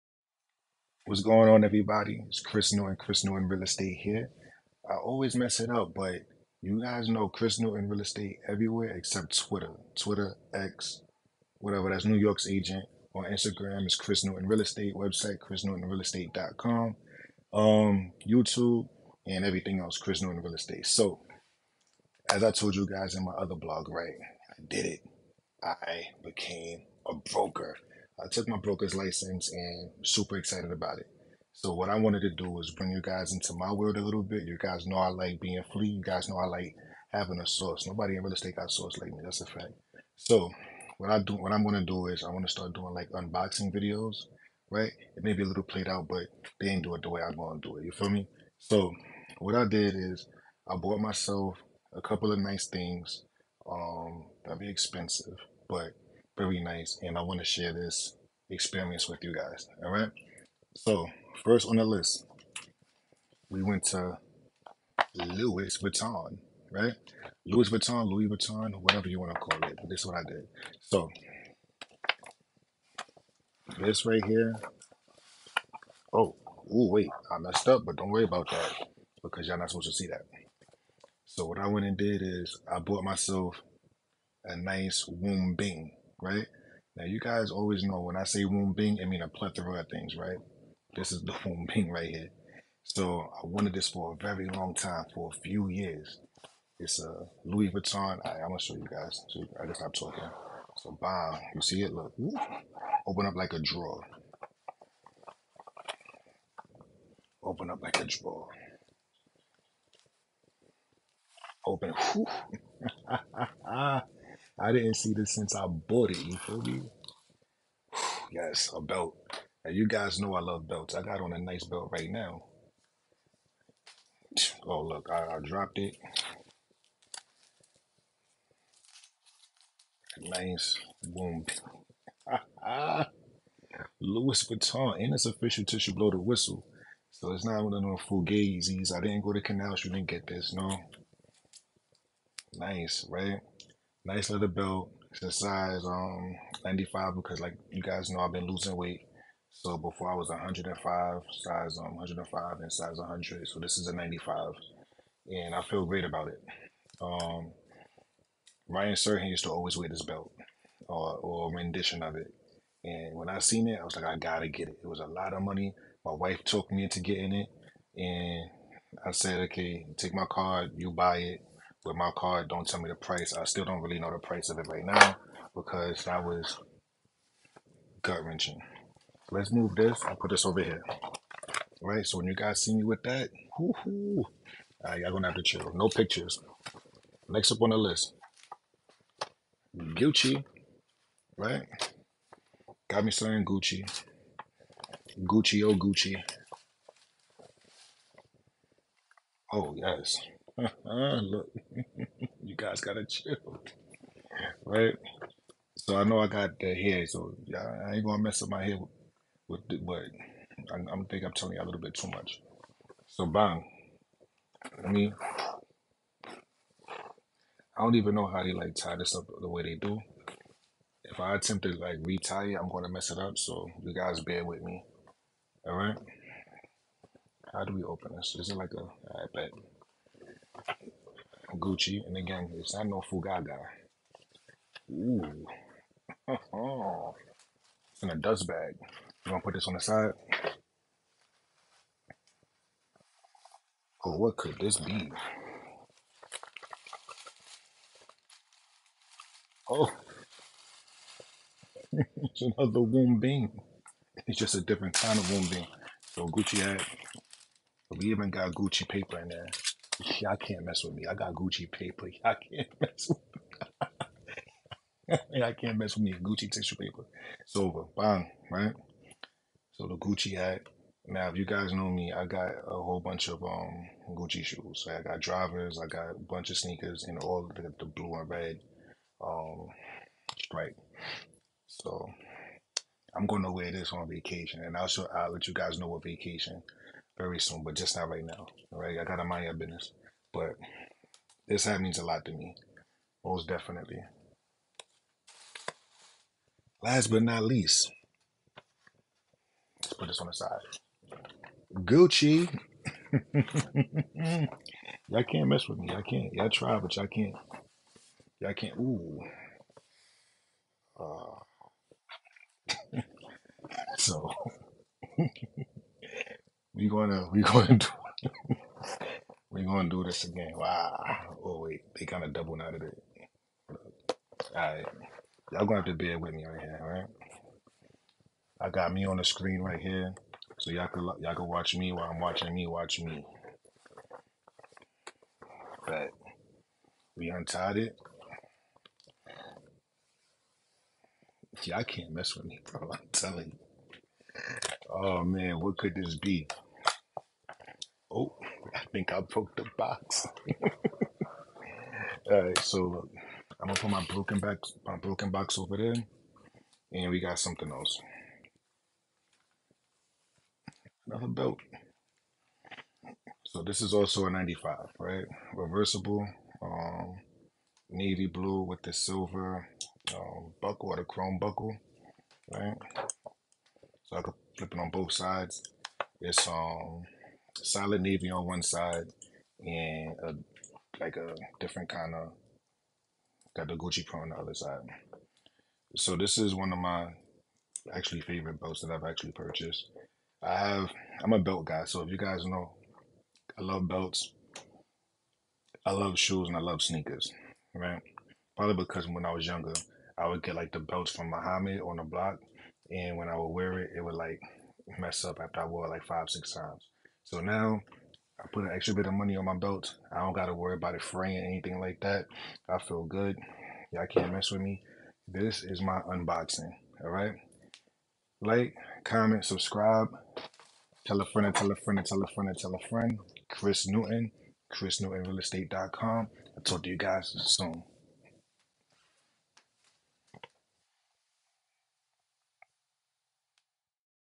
Louis Vuitton & Gucci Unboxing
Welcome To Chris Newton Real Estate. In this video, New York Real Estate Broker Chris Newton does an unboxing video to treat himself for his accomplishments.